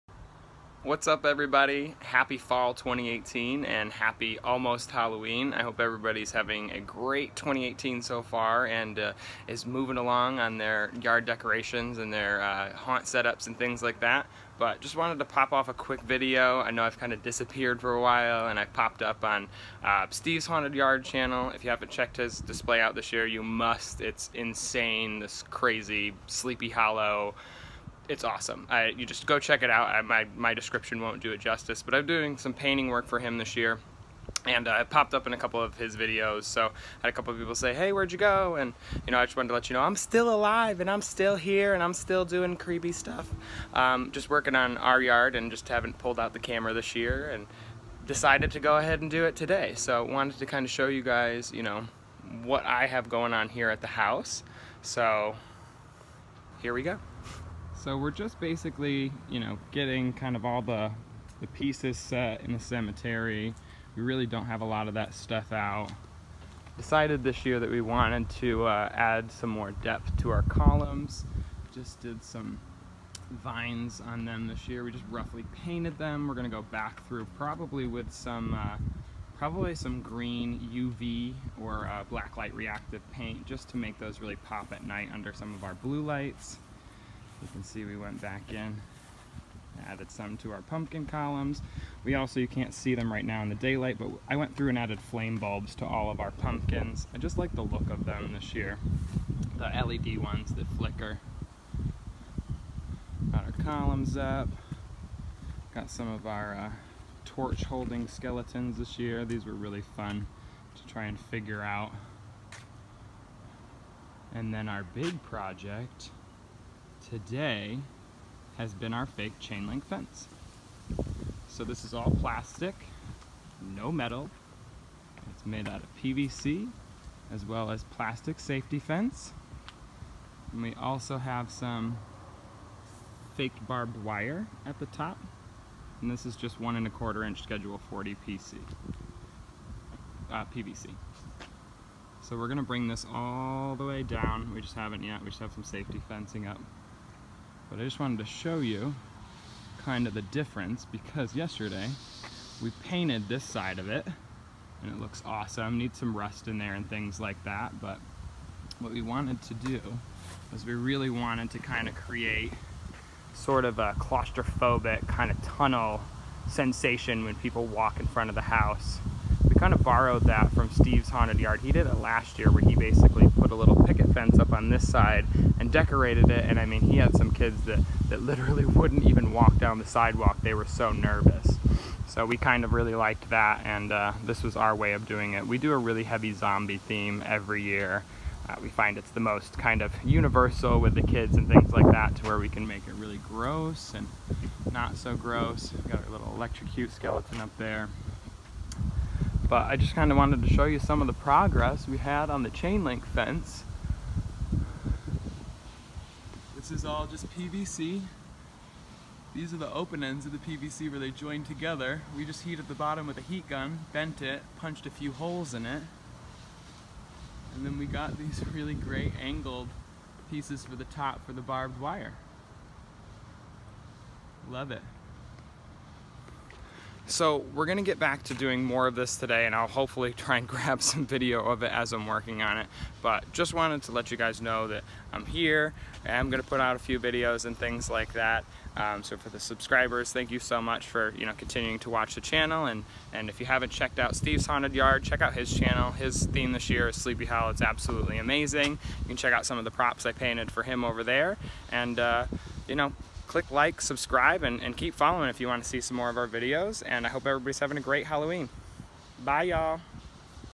We’re Back... Halloween 2018 Set-up and Fake Chainlink Fence
We are back y’all! Hope everyone is having a great 2018. We are busy setting up our Halloween Yard display and wanted to share what we have been up to.

This Faux / Fake chainlink we created was all plastic and looks great!